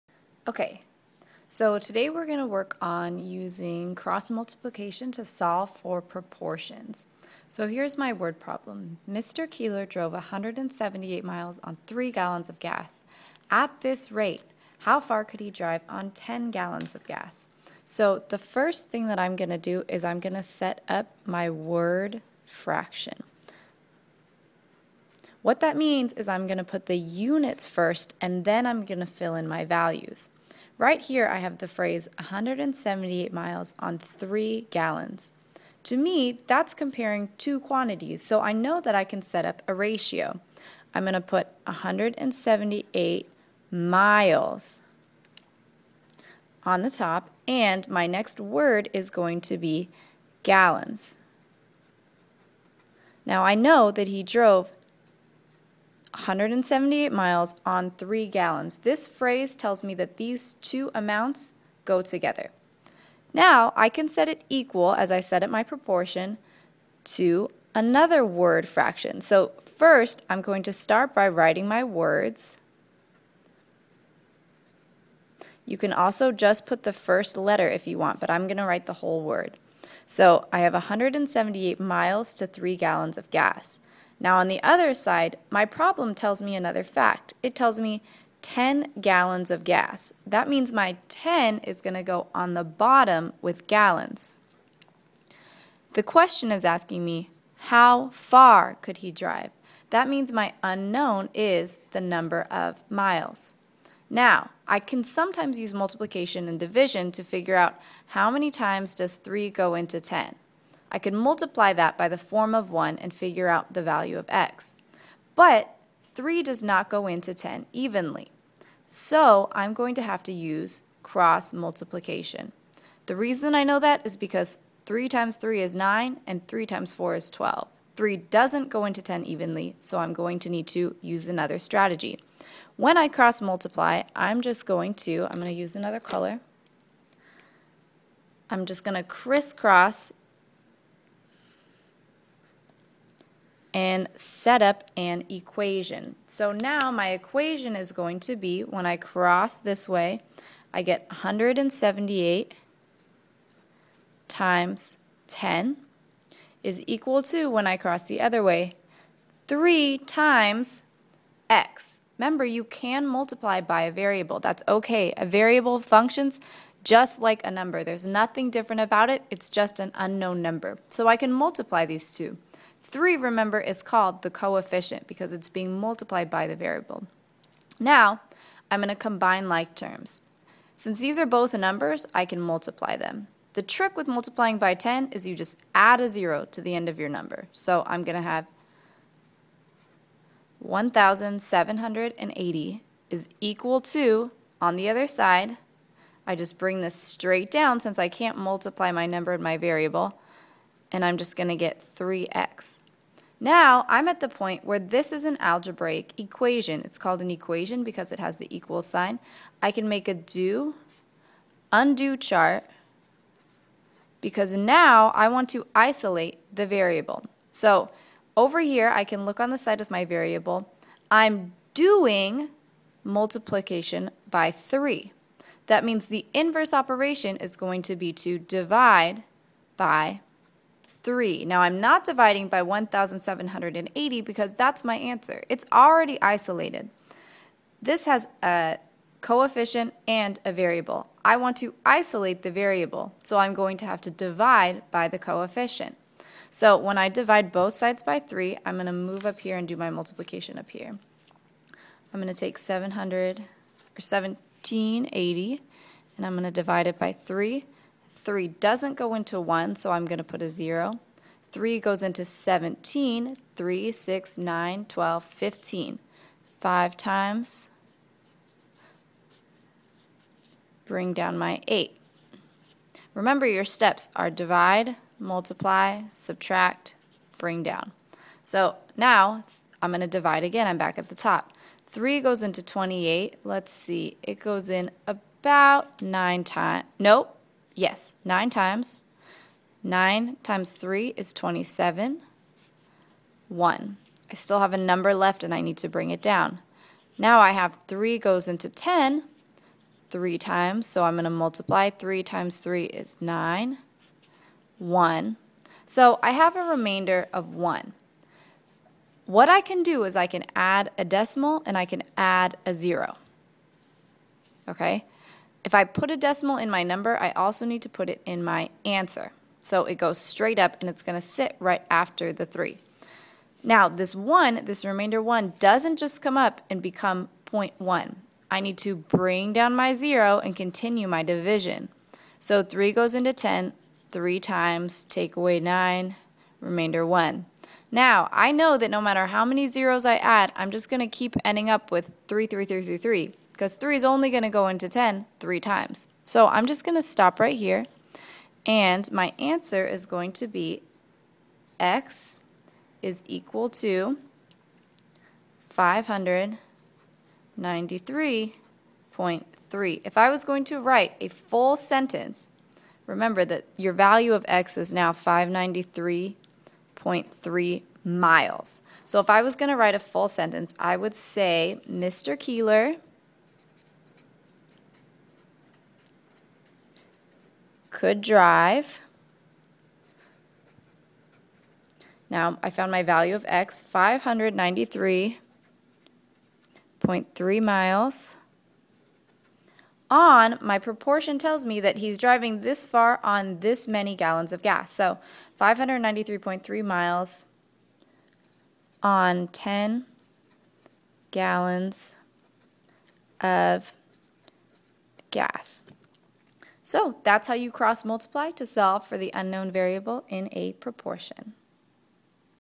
Equivalent Rate Word Problem - Solve with a Proportion
Using cross-multiplying and a proportion to solve for an equivalent rate in a word problem.

If you like this video, please leave a "thumbs up" or a comment!